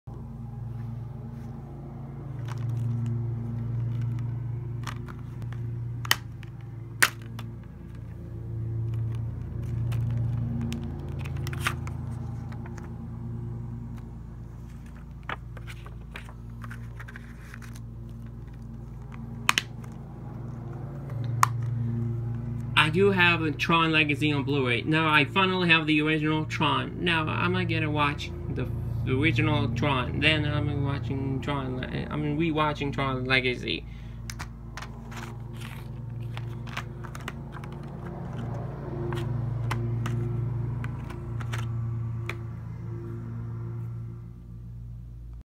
Quick Blu-Ray Update for October 30th, 2020+Bonus Update
I got 1 Blu-Ray at Wal-Mart and few stuff at Dollar Tree today!